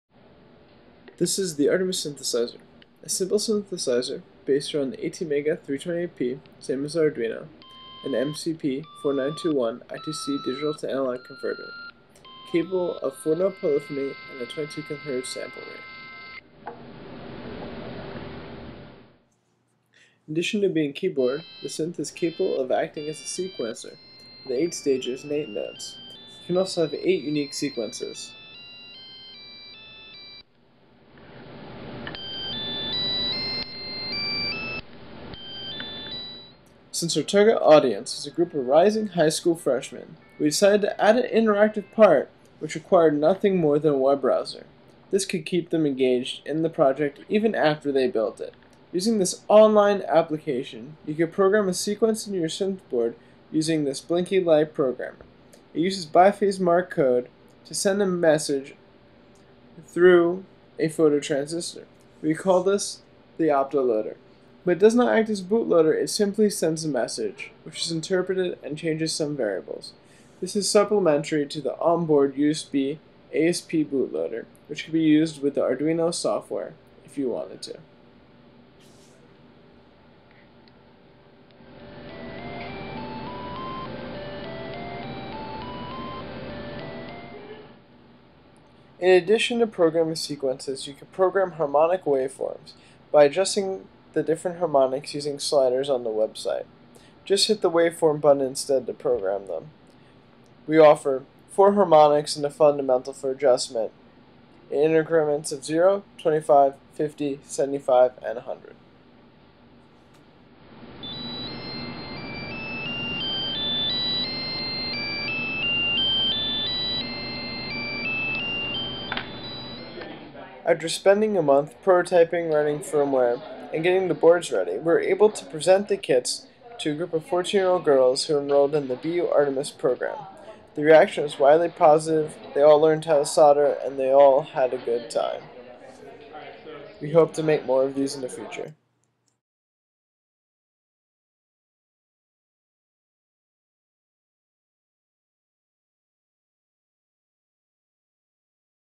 Artemis Synthesizer Overview
A basic overview and presentation of the Artemis Synthesizer built in about a month as a kit for a group of 24 rising high school freshmen enrolled in Boston University's Women in Technology outreach program called "Artemis". Made by the Boston University EDF headed by Christopher Woodall (cjwoodall.com and happyrobotlabs.com), and with the support of the BU Physics department.